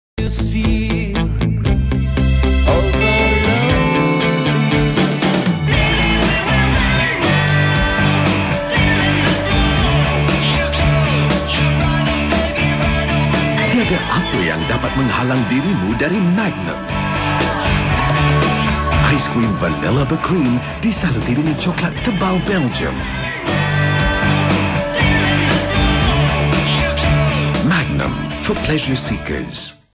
[TVC] Magnum Classic Ice Cream - For Pleasure Seekers
A Malaysian TV commercial.
Year: 2012